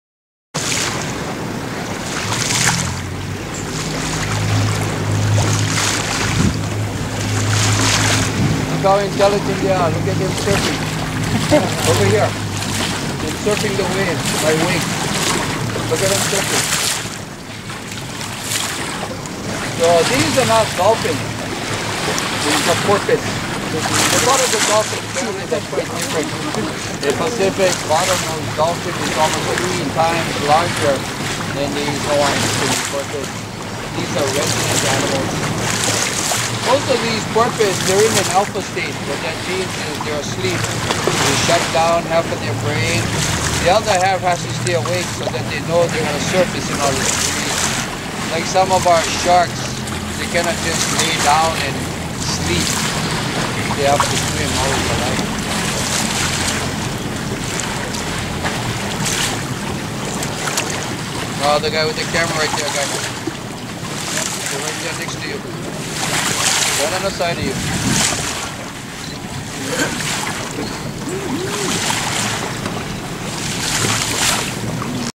Catamaran Kahanu's Discovery 17 March 2011
Captain locates pod of porpoise. Boats converge.